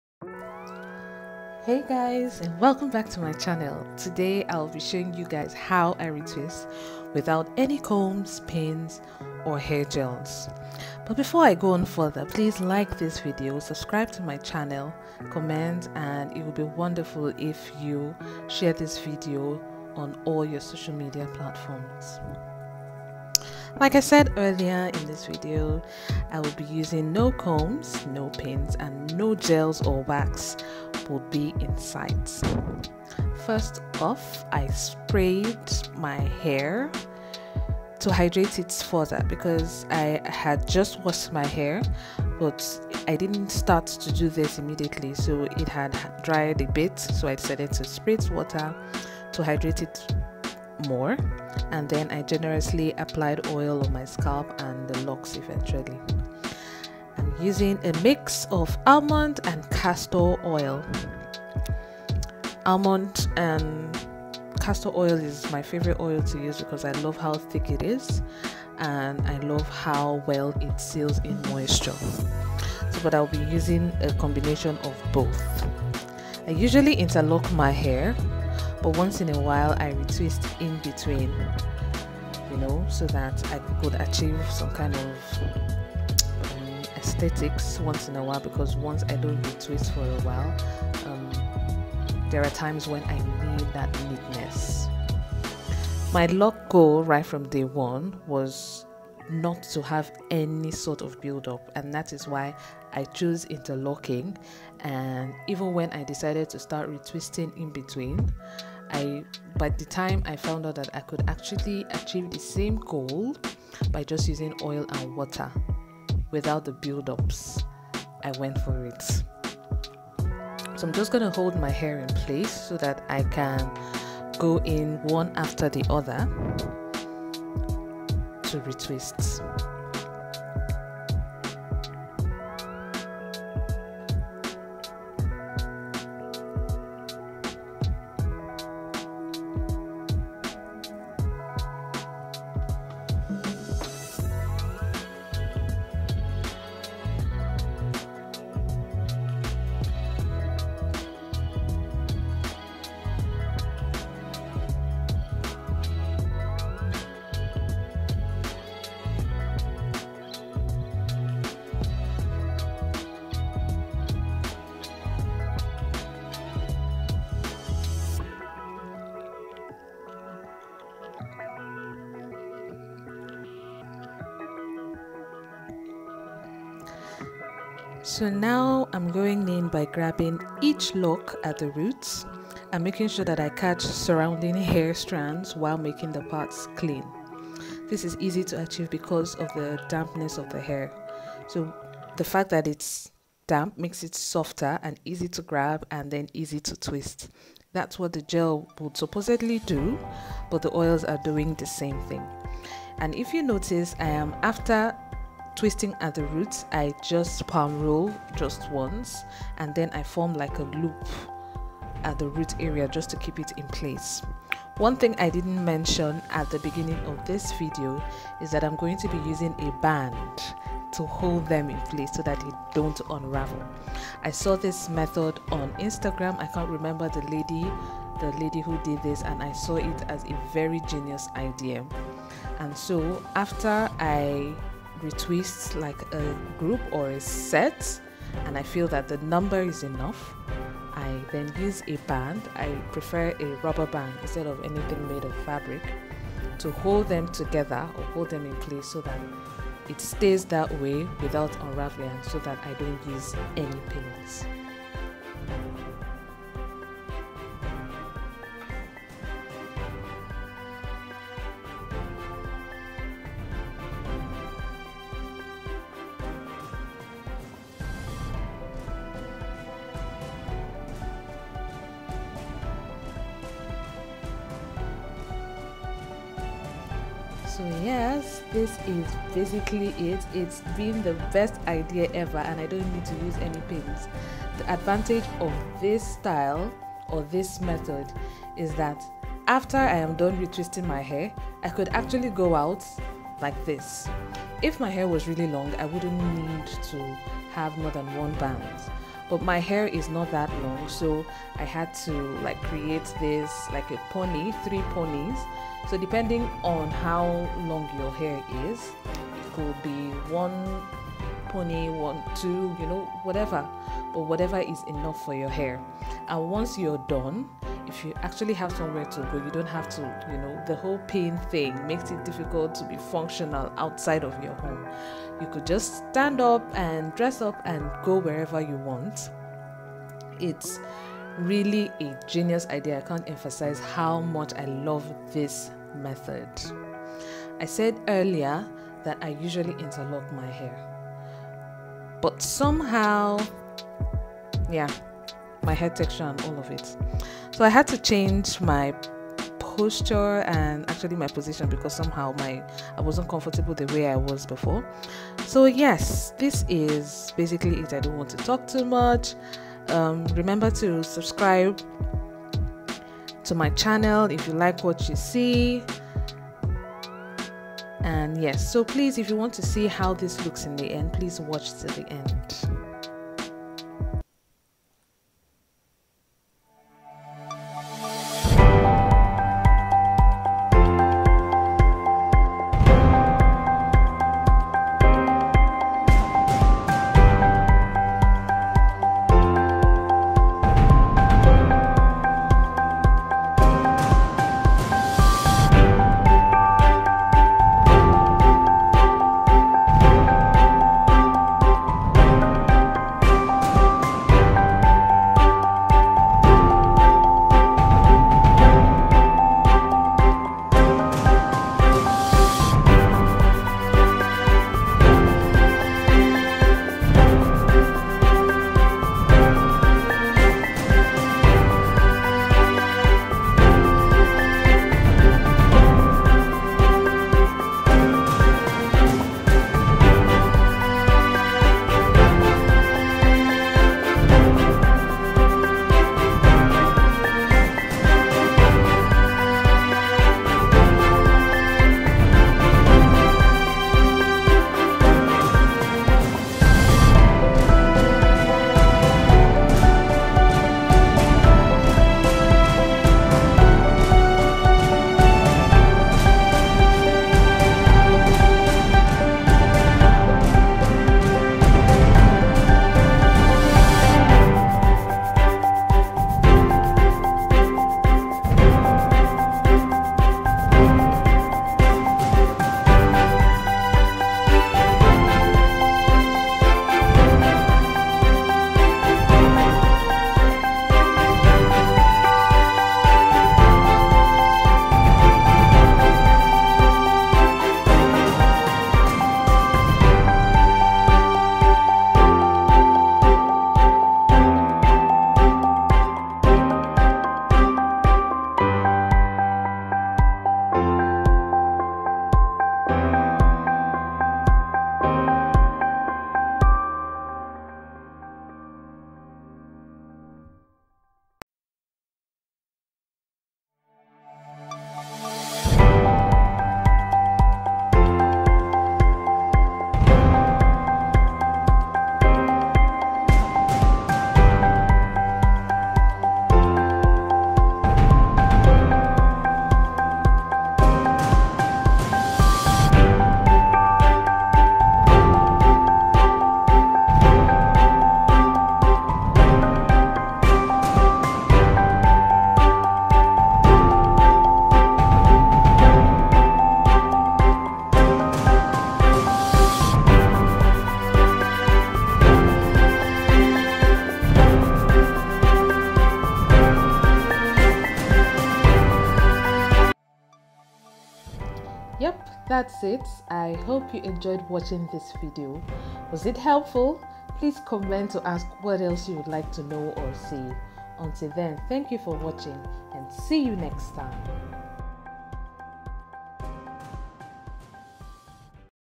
How to Retwist Locs. No Gels!! No Hair Clips!!!
Retwisting with gels are wonderful but loc  buildups are always a nighmare. Here's a better way to Retwist your hair to avoid buildups.